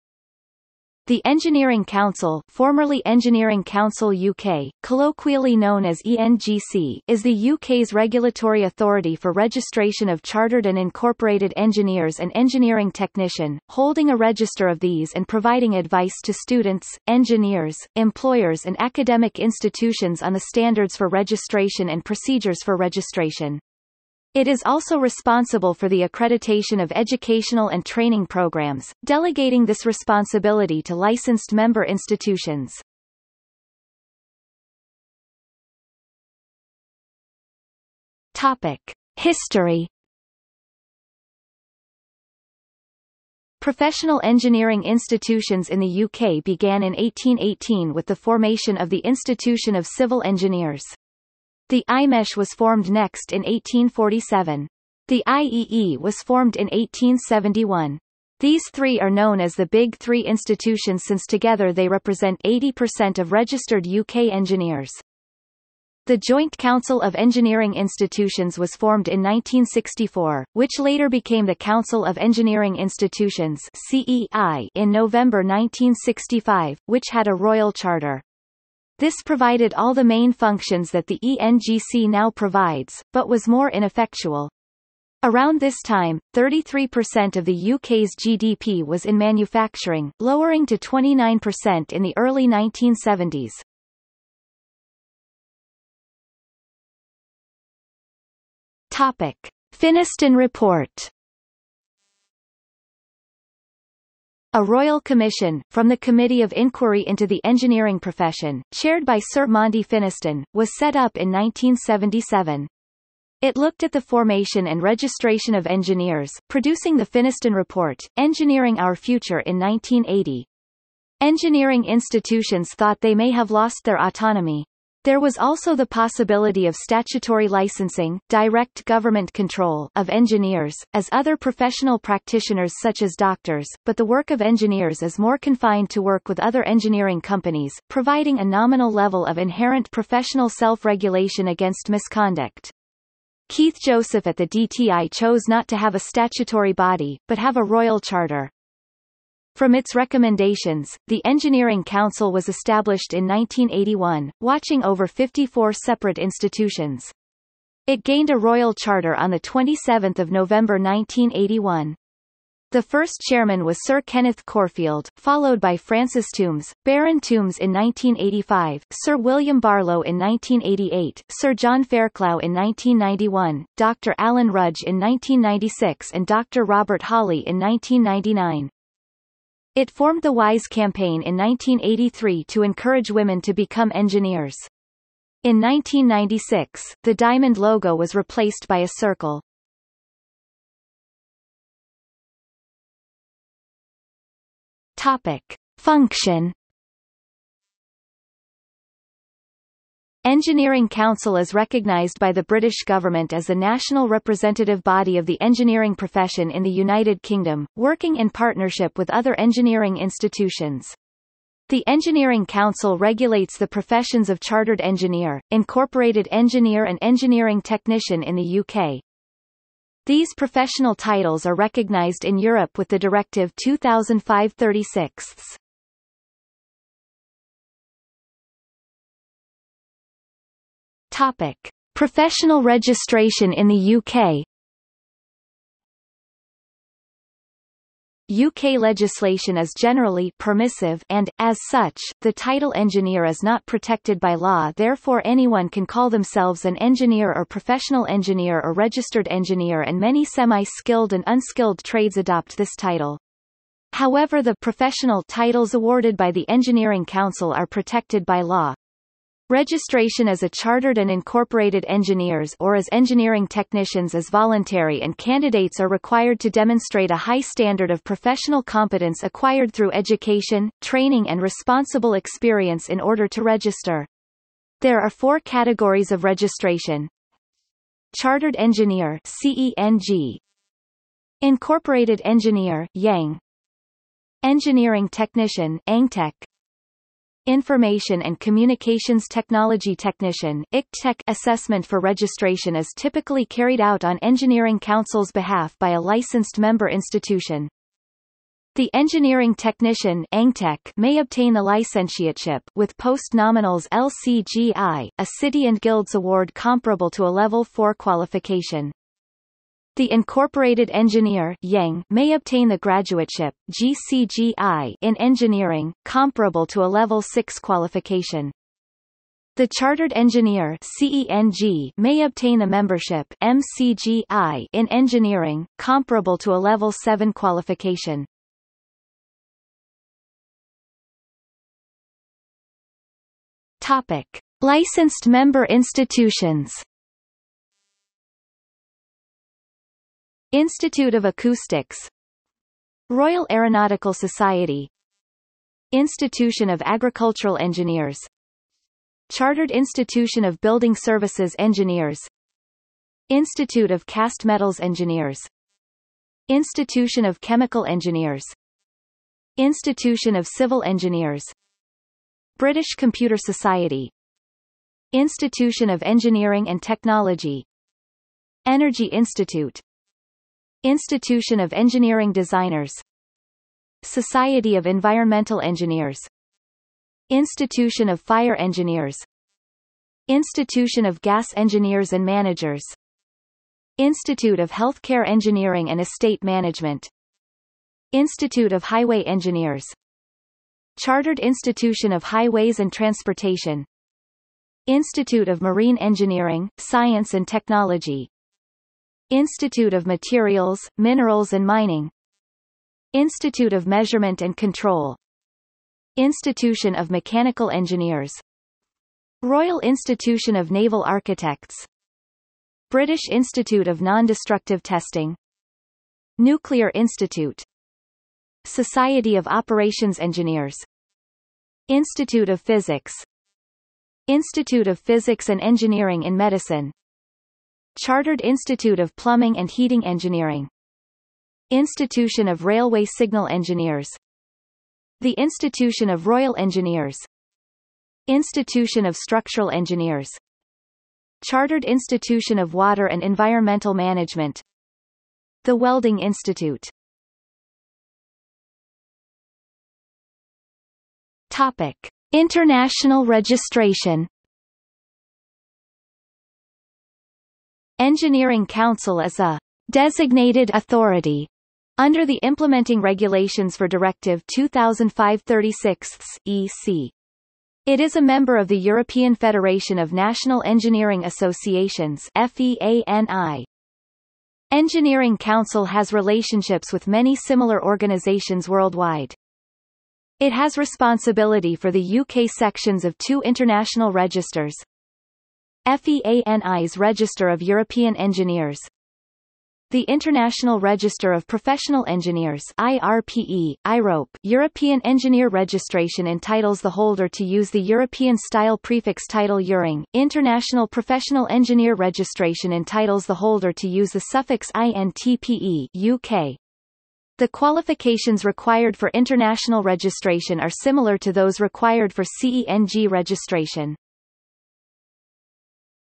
Engineering Council | Wikipedia audio article
00:00:34 1 History
00:01:35 1.1 Finniston report
00:03:11 2 Function
00:03:47 3 Professional registration in the UK
00:05:30 4 Licensed member institutions
00:07:27 5 International registration

- Socrates

SUMMARY
The Engineering Council (formerly Engineering Council UK; colloquially known as EngC) is the UK's regulatory authority for registration of Chartered and Incorporated engineers and engineering technician, holding a register of these and providing advice to students, engineers, employers and academic institutions on the standards for registration and procedures for registration. It is also responsible for the accreditation of educational and training programs, delegating this responsibility to licensed member institutions.